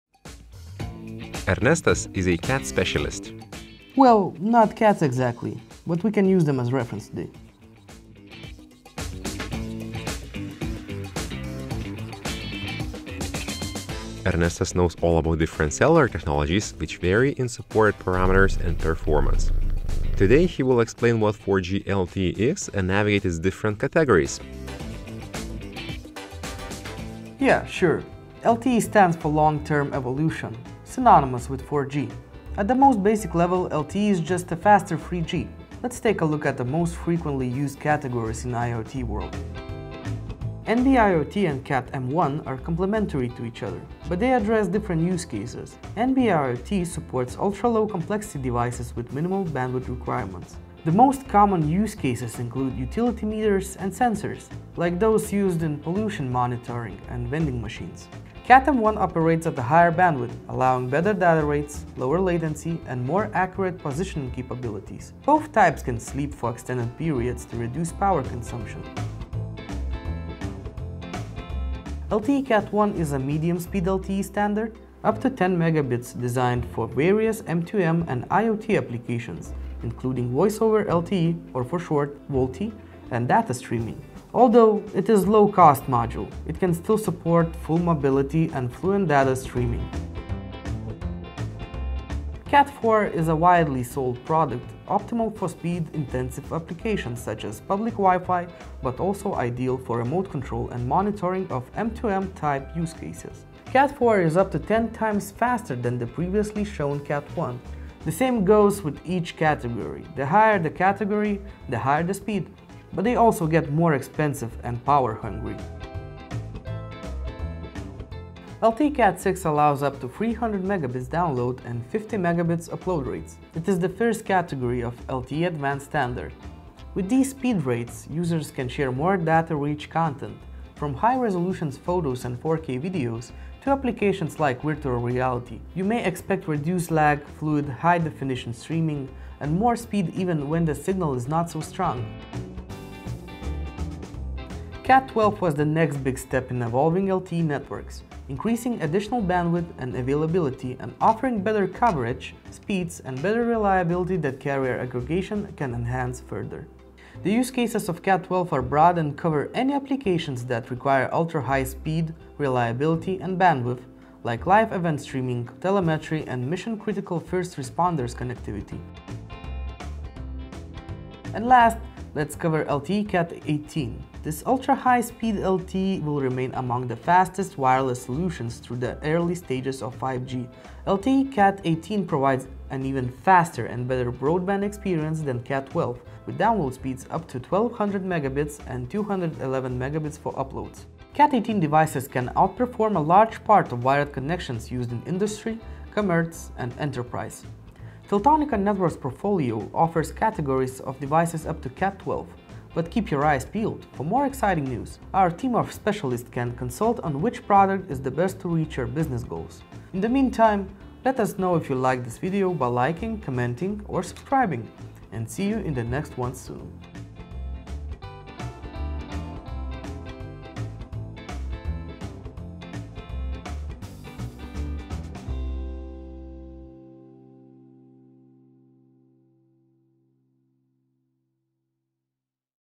4G LTE Categories (a.k.a CATs 😽) – What’s the difference?
Today we invite you to learn how to navigate among different LTE categories (aka CATs) with the assistance of cats themselves!

Timestamps:
00:00 Intro
00:28 What is LTE?
00:39 NB-IoT & CAT-M1
01:30 LTE CAT 4
01:56 LTE CAT 6
02:23 LTE CAT 12
02:49 LTE CAT 18
03:19 Outro